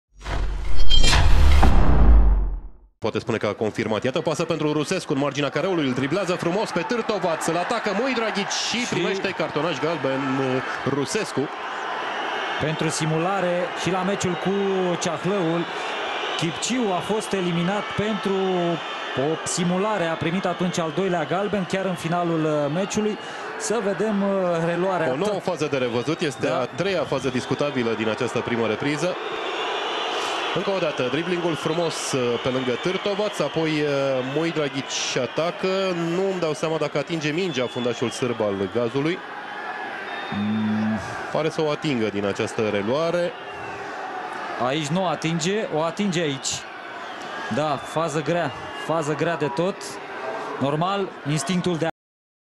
Rusescu primeşte galben pentru simulare cu Gaz Metan
Steaua cere lovitură de la 11 metri la pătrunderea lui Rusescu, însă arbitrul îl avertizează pentru simulare pe atacantul gazdelor.